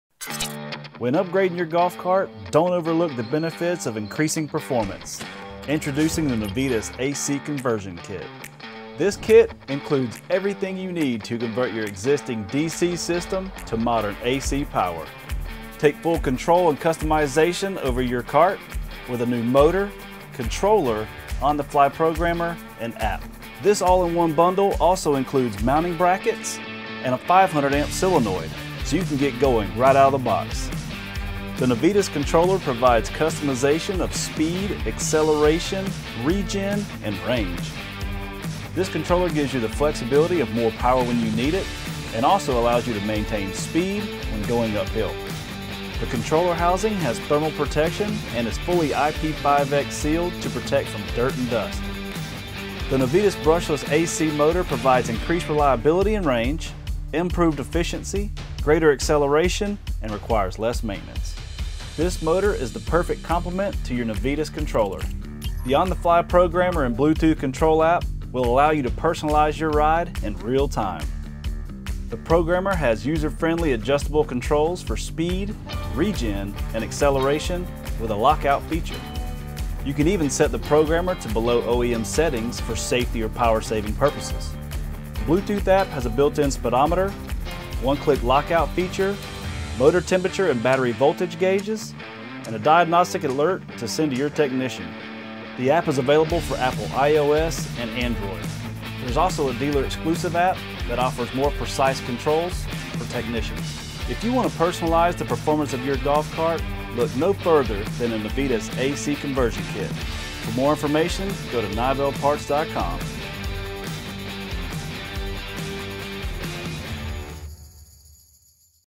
AC Conversion Kit || Nivel Golf Cart Accessories
This Navitas 600A DC to AC conversion kit will increase the overall efficacy of your cart. Noticeable improvements in range, battery life, and performance (speed & torque). The On-The-Fly Programmer allows you to personalize your driving experience in real-time. Adjust your top speed, braking, acceleration, and battery driving range all with the turn of a knob. With the newly added Bluetooth feature, you can now turn your phone into a vehicle instrument panel. The dashboard app allows the vehicle's owner to remotely lock their vehicle, monitor usage and performance or notify their dealers with diagnostic information.